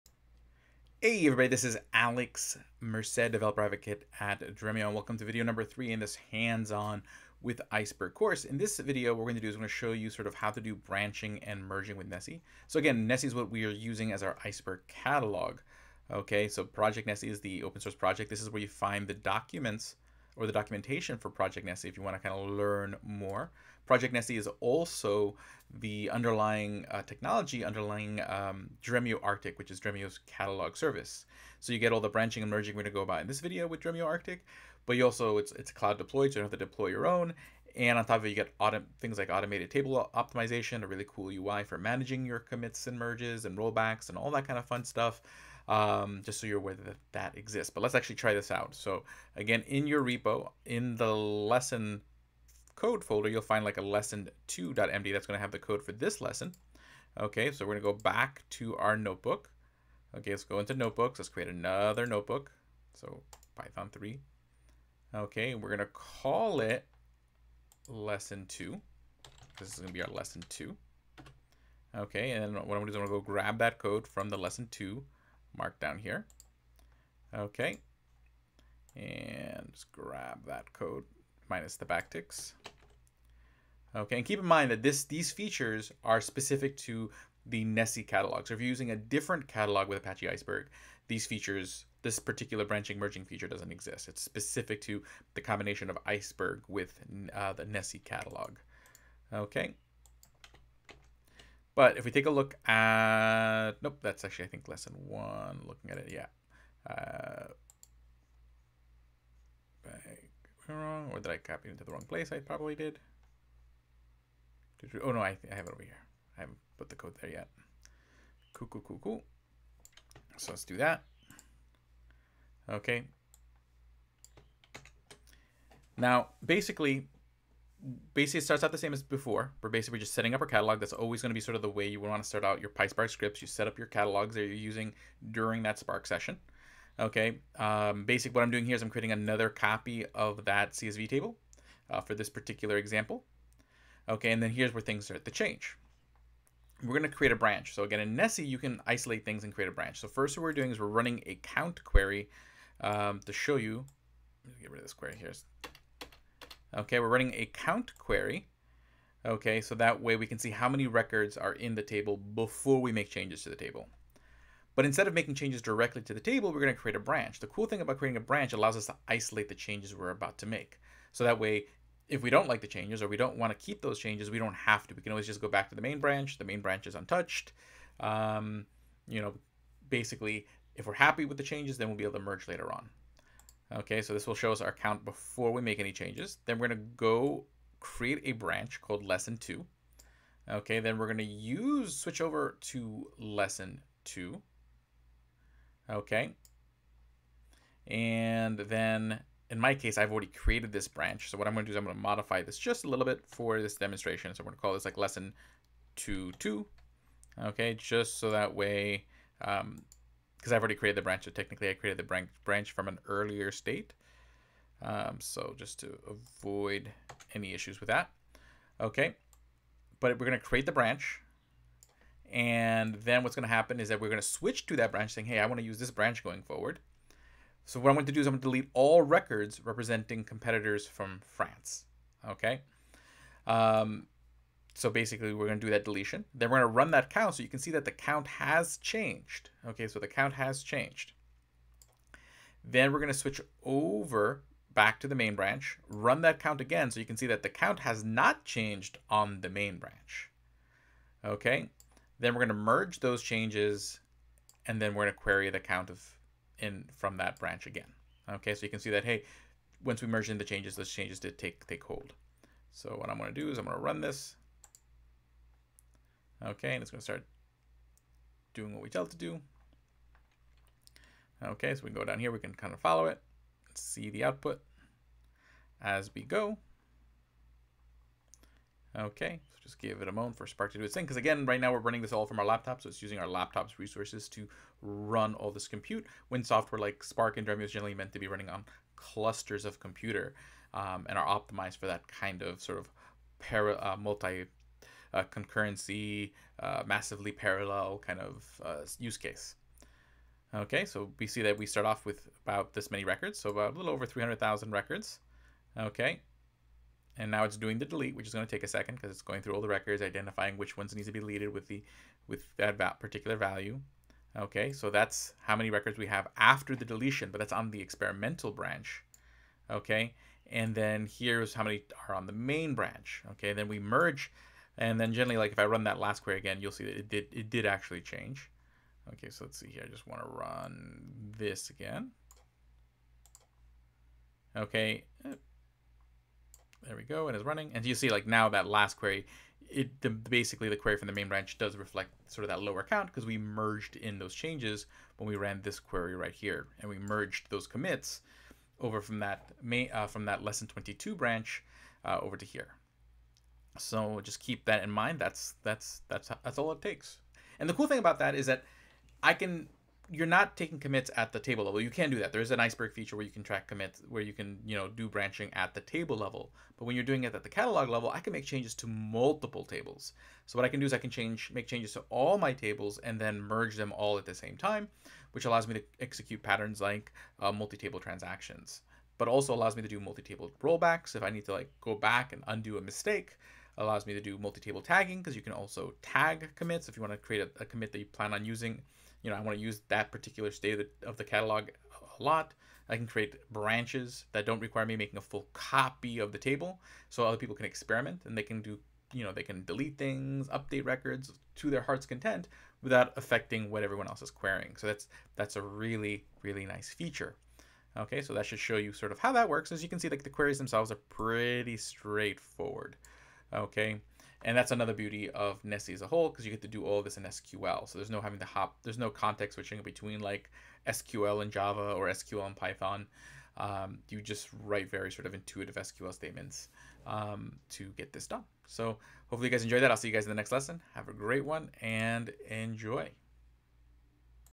Hands-On Intro to Apache Iceberg - 3 - Nessie Catalog Branching/Merging in Apache Spark
In this video series Dremio Developer Advocate demonstrates how to work with Apache Icberg tables using Spark & Dremio with a Nessie catalog.